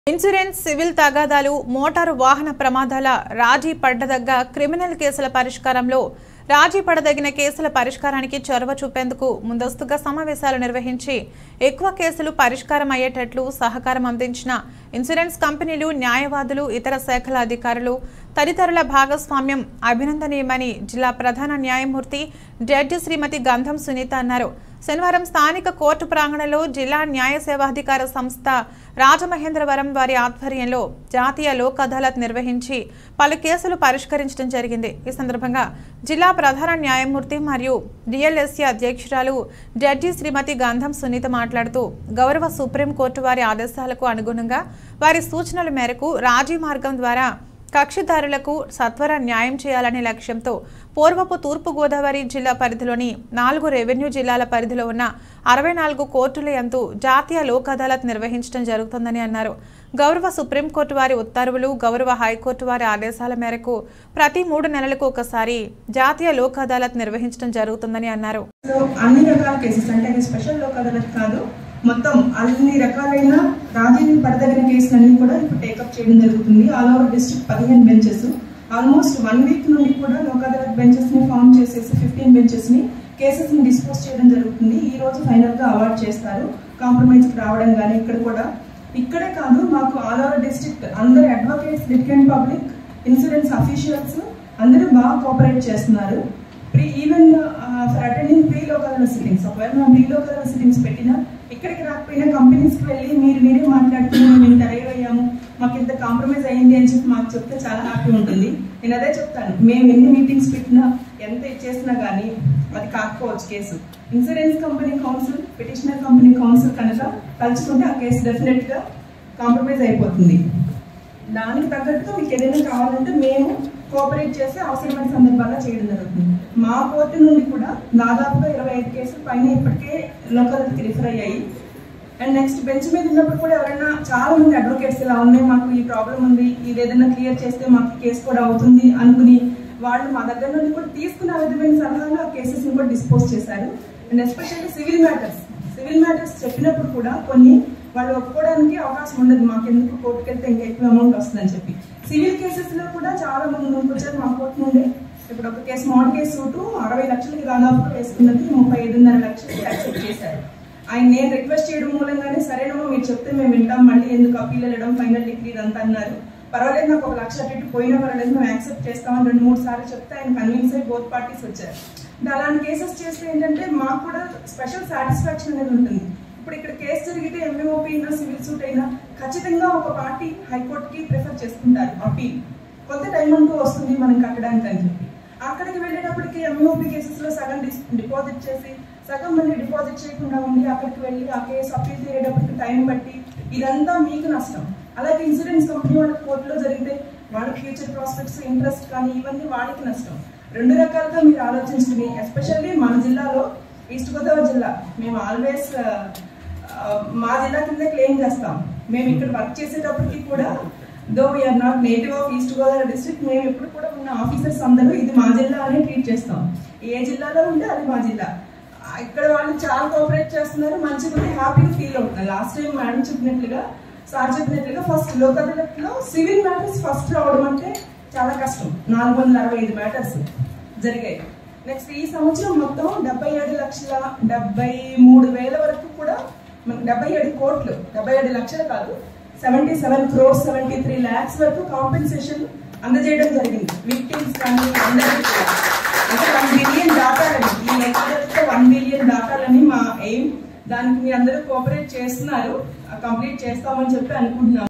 ABC NEWS TELUGU//రాజీ పడతగ్గ క్రిమినల్కేసుల పరిష్కారమయ్యేటట్లు సహకారమందించిన అందరికి అభినందనలు.
పత్రికా ప్రకటన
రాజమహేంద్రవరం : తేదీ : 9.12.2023
** శనివారం జరిగిన జాతీయ లోక్ అదాలత్ సందర్భంగా రాజీ మార్గంలో అవార్డుల జారీ
** ఉమ్మడి తూర్పు గోదావరి జిల్లాలో  కోర్టుల పరిధిలో 47  బెంచ్ లు నిర్వహణ
** 1 నెల రోజుల ముందు నుంచే కేసుల పరిష్కారం కోసం చర్యలు
** ఈరోజు  కేసులు పరిష్కారం రూ. కోట్ల లకు అవార్డ్ జారీ
- జిల్లా ప్రధాన జిల్లా జడ్జి   శ్రీమతి గంధం సునీత
ఇన్సూరెన్స్, సివిల్ తగాదాలు, మోటారు వాహన ప్రమాదాల, రాజీ పడతగ్గ క్రిమినల్ కేసుల పరిష్కారం లో రాజీ పడదగిన  కేసుల పరిష్కారానికి  చొరవ చూపేందుకు ముందస్తూగా సమావేశాలు నిర్వహించి, ఎక్కువ కేసులు పరిష్కారం అయ్యేటట్లు సహకారం అందించిన ఇన్సూరెన్స్ కంపెనీలు, న్యాయవాదులు, ఇతర శాఖల అధికారులు, తదితరుల భాగస్వామ్యం అభినందనీయమని  జిల్లా ప్రధాన న్యాయమూర్తి జడ్జి  శ్రీమతి గంధం  సునీత అన్నారు.
శనివారం స్థానిక కోర్టు ప్రాంగణంలో  జిల్లా న్యాయ సేవాధికార సంస్థ, రాజమహేంద్రవరం వారి  ఆధ్వర్యంలో జాతీయ  లోక్ అదాలత్ నిర్వహించి పలు కేసులు పరిష్కరించడం జరిగింది.
ఈ సందర్భంగా జిల్లా ప్రధాన న్యాయమూర్తి మరియు డి ఎల్ ఎస్ ఎ అధ్యక్షురాలు జడ్జి శ్రీమతి గంధం సునీత  మాట్లాడుతూ, గౌరవ సుప్రీం కోర్టు వారి ఆదేశాలకు అనుగుణంగా వారి సూచనలు మేరకు రాజీ మార్గం ద్వారా కక్షి దారులకు సత్వర న్యాయం చేయాలనే లక్ష్యంతో పూర్వపు తూర్పు గోదావరి జిల్లా పరిధిలోని నాలుగు రెవెన్యూ జిల్లాల పరిధిలో ఉన్న 64 కోర్టుల యందు జాతీయ లోక్ అదాలత్ నిర్వహించడం జరుగు తోందని అన్నారు.
గౌరవ సుప్రీం కోర్టు వారి ఉత్తర్వులు, గౌరవ హై కోర్టు వారి ఆదేశాల మేరకు ప్రతి మూడు నెలలకు ఒకసారి జాతీయ లోక్ అదాలత్ నిర్వహించడం జరుగుతోందని అన్నారు.  మోటార్ ప్రమాదాల కారణంగా పరిహారం చెల్లింపులో ఆలస్యం నివారణ చేసే విధానం లో  లోక్ అదాలత్ నిర్వహించి కక్షి దారులకు సత్వర న్యాయం కోసం నిర్వహించడం ముఖ్య ఉద్దేశ్యం అన్నారు. తద్వారా ఇటువంటి లోక్ అదాలత్ లు ఇరు పక్షాలకు ఎంతో మేలు చేకూరుతుందని అన్నారు. ఆమేరకు ముందుగా సమావేశం నిర్వహించి ఇరుపక్షాలను  రాజీ చేయడం ద్వారా ఈరోజు ఆమేరకు అవార్డు జారీ చేశామన్నారు.
అన్ని రకాలైన రాజీ పడతగ్గ కేసులను జాతీయ లోక్ అదాలత్ లో రాజీ మార్గం ద్వారా పరిహారం చెల్లించాలని అవార్డ్ ఇవ్వడం జరిగిందన్నారు . కొన్ని ఇన్సూరెన్స్ కంపెనీలు మోడల్ చెక్కు పంపిణీ చేస్తామని ముందుకు రావడం అభినందనీయం అన్నారు. జాతీయ లోక్ అదాలత్ ద్వారా తక్షణం రాజీ చెయ్యడం వల్ల ఉభయులకు సత్వర న్యాయం జరుగుతుందని, ఇందు వల్ల ఇన్సూరెన్స్ కంపెనీ లకు కూడా ఎంతో మేలు జరిగే అంశం అని గంధం సునీత పేర్కొన్నారు. ప్రజల్లో, కక్షి దారుల్లో లోక్ అదాలత్ లపై మరింతగా అవగాహన పెంచి సత్వర న్యాయం జరిగేలా చర్యలు తీసుకోవడం లో సామాజిక బాధ్యత అని స్పష్టం చేశారు. ఇరు పక్షాల మధ్య రాజీ మార్గం ద్వారా ఈరోజు నిర్వహించే జాతీయ  లోక్ అదాలత్ బెంచ్ కు వొచ్చిన కేసులకు పరిష్కారం చూపి ఆమేరకు అవార్డ్ ను ప్రకటించినట్లు పేర్కొన్నారు.
ఈ ఏడాది 4 సార్లు జాతీయ లోక్ అదాలత్,  3 సార్లు ప్రత్యేక లోక్ అదాలత్ నిర్వహించడం ద్వారా కేసుల పరిష్కారం ద్వారా కక్షి దారులకు సత్వర  న్యాయం జరిగేలా న్యాయ మూర్తులు, కోర్టులు పనిచేస్తాయని వారు పేర్కొన్నారు.
పరిష్కారం చూపిన కొన్ని తీర్పులు,:
మోటారు వాహనాల కేసులకు సంబంధించి  ఎంవిఒపి నెం. 549/2022  కేసులో పిటీషనర్  జుత్తుక బాలక్రిష్ణ మరియు ఇద్దరికి నేషనల్ ఇన్సూరెన్స్ ద్వారా రూ. 50 లక్షల పరిహారం,  ఎంవిఒపి నెం. 362/2021  కేసులో పిటీషనర్ దుళ్ళ రాజ్ కుమార్ మరియు ఇద్దరికి చోల మండళం జనరల్ ఇన్సూరెన్స్ ద్వారా రు.38 లక్షలు, ఎంవిఒపి నెం. 674/2022  కేసులో పిటీషనర్ ఎద్దాడ సత్తిబాబు కి న్యూ ఇండియా ఎష్యూరెన్స్  కంపెనీ  ద్వారా రూ. 10 లక్షల పరిహారం, ఎంవిఒపి నెం.683/2022  కేసులో పిటీషనర్ తిలపాక లక్ష్మి మరియు నలుగురికి నేషనల్ ఇన్సూరెన్స్ ద్వారా రూ. 9 లక్షల 70 వేలు పరిహారం,  ఎంవిఒపి నెం. 24/2023  కేసులో పిటీషనర్ తాడి శ్రీధర్ కి  నేషనల్ ఇన్సూరెన్స్ ద్వారా రూ. 26 లక్షల పరిహారం అందించడం జరిగింది.
ఈ జాతీయ  లోక్ అదాలత్ లో 1 వ అదనపు జిల్లా జడ్జి  శ్రీ పి. ఆర్. రాజీవ్,  జిల్లా న్యాయ సేవాధి కార సంస్థ కార్యదర్శి మరియు సీనియర్ సివిల్ జడ్జి శ్రీమతి కె. ప్రత్యూష కుమారి,  బార్ అసోసియేషన్ ఉపాధ్యక్షులు గేదెల వెంకటేశ్వరరావు, ఇతర న్యాయ మూర్తులు, ఇన్సూరెన్స్ కంపెనీ ప్రతినిధులు, కక్షిదారులు, అధికారులు తదితరులు పాల్గొన్నారు.

జిల్లా సమాచార పౌర సంబంధాల అధికారి, రాజమహేంద్రవరం వారిచే జారీ.